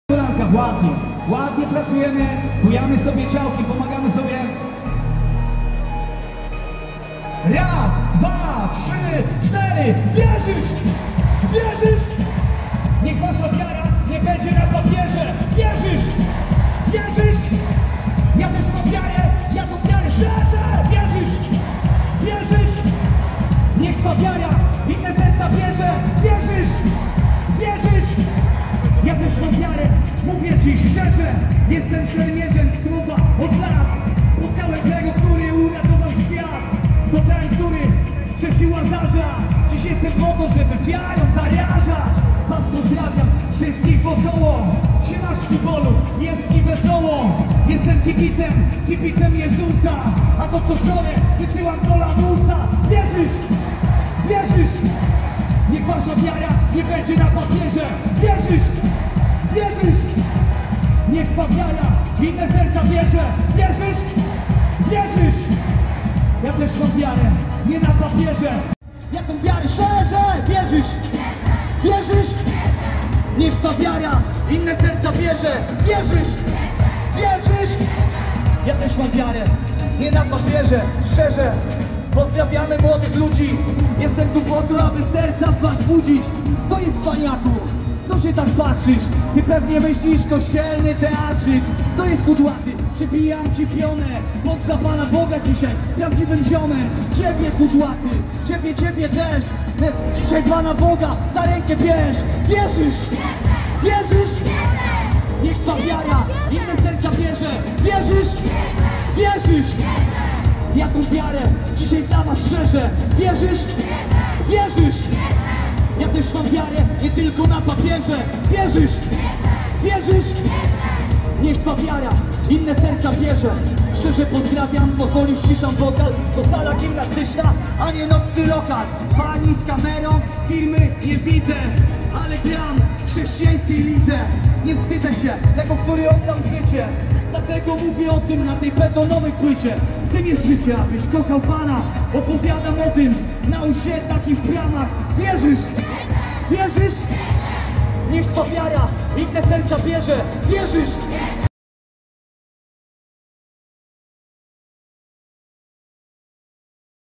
Wierzysz? Wierzę! I Arena Młodych I 2022
Relacja z największego wydarzenia ewangelizacyjnego dla młodzieży w Wielkim Poście 2022.